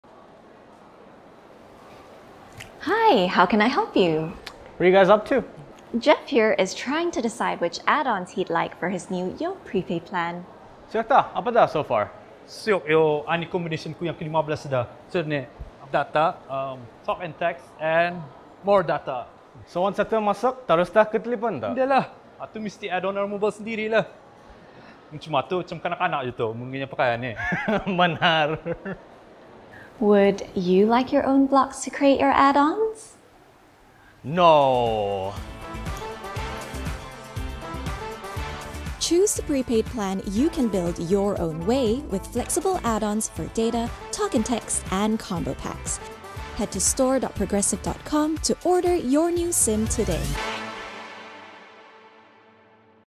Build Your Own Prepaid
It'ssss Charlotte 🎶 And she's back with an important message - Get the new customisable YO! Prepaid and builditYOway

With flexible Add-ons, the most data per dollar, bonus free data, a free sim and more for only $10 with $10 preloaded credit, there's so much to love about YO!

Head to store.progresif.com to order for free delivery today.